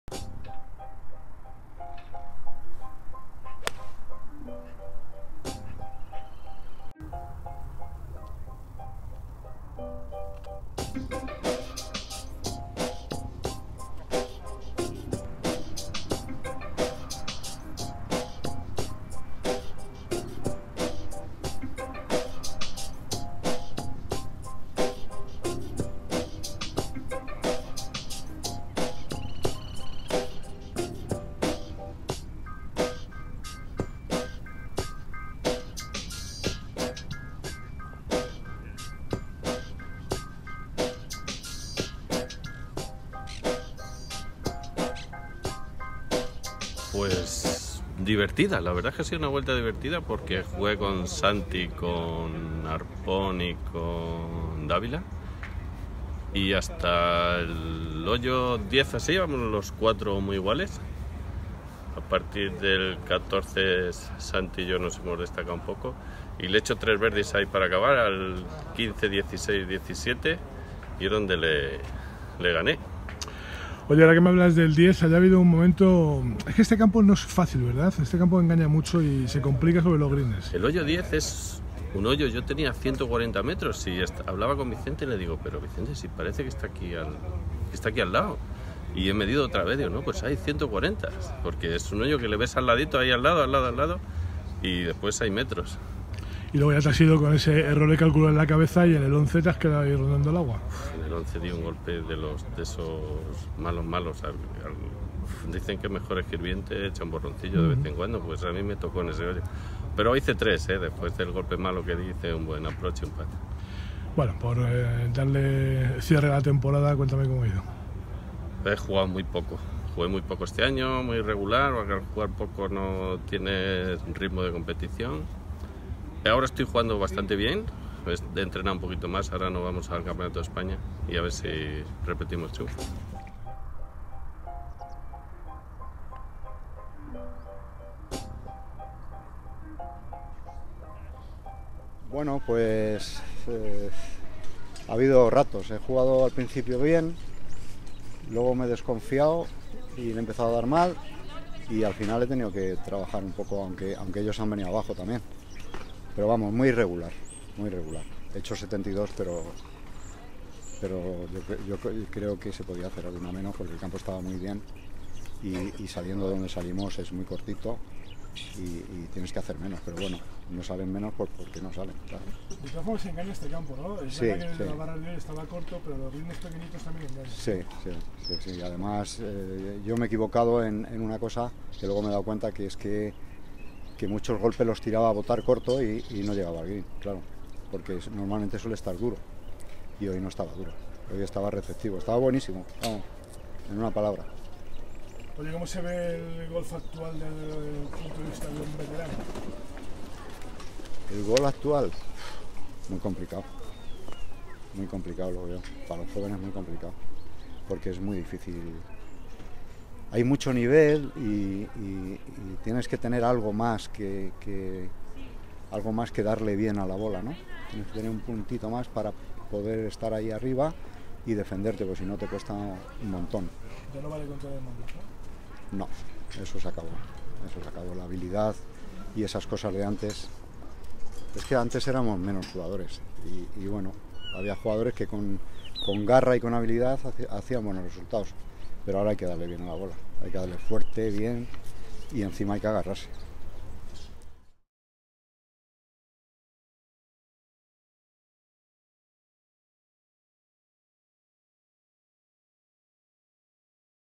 Campeonato de Madrid de Profesionales Sénior
Manuel Moreno y Manuel Montes, campeones de Madrid sénior y supersenior, respectivamente